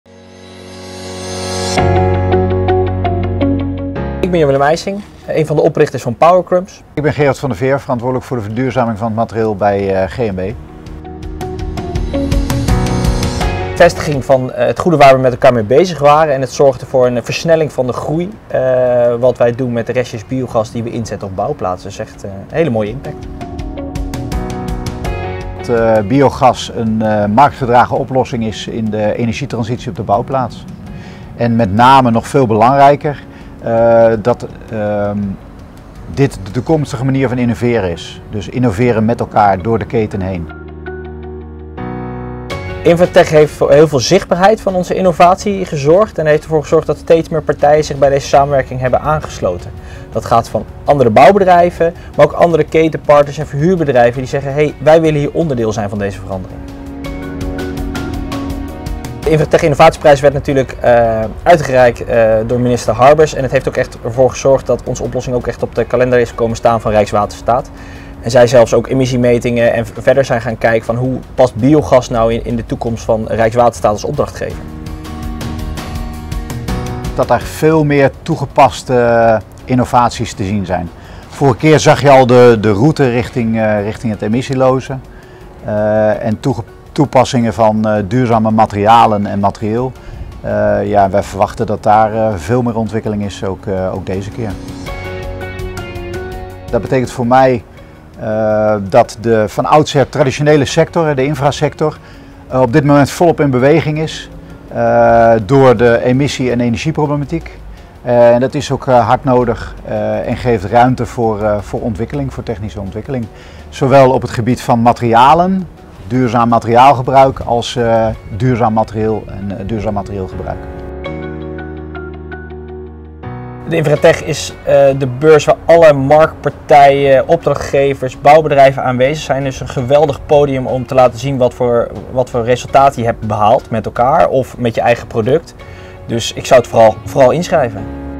InfraTech Innovatieprijs Winnaar 2023 blikt terug
GMB blikt terug op het winnen van de Innovatieprijs in 2023 en kijkt alvast vooruit naar de 2025 editie met het nieuwe thema Ruimte voor Ontwikkeling. Wilt u dit jaar meedoen met de Innovatieprijs?